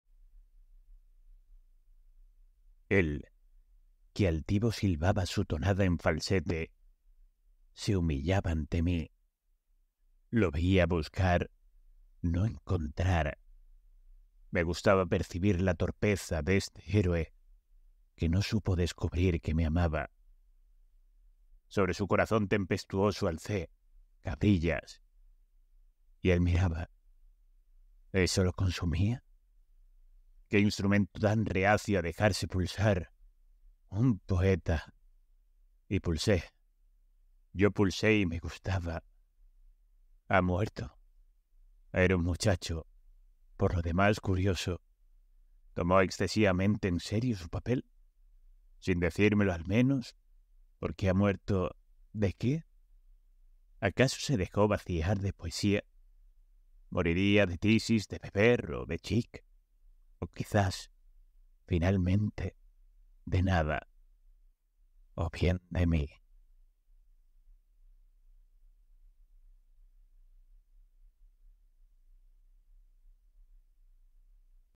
Pobre muchacho - Tristán Corbière
Él, que altivo silbaba su tonada en falsete,
Se humillaba ante mí: lo veía buscar…
No encontrar… me gustaba percibir la torpeza
De este héroe que no supo descubrir que me amaba.

Sobre su corazón tempestuoso alcé
Cabrillas. Él miraba… ¿Eso lo consumía?
¡Qué instrumento tan reacio a dejarse pulsar,
Un poeta!… Y pulsé. Yo pulsé y me gustaba.

¿Ha muerto?… Era un muchacho, por lo demás curioso.
¿Tomó excesivamente en serio su papel?
Sin decírmelo… al menos. –Porque ha muerto, ¿de qué?…

¿Acaso se dejó vaciar de poesía?…
¿Moriría de tisis, de beber o de chic?
O quizás, finalmente: de nada…
O bien de Mí.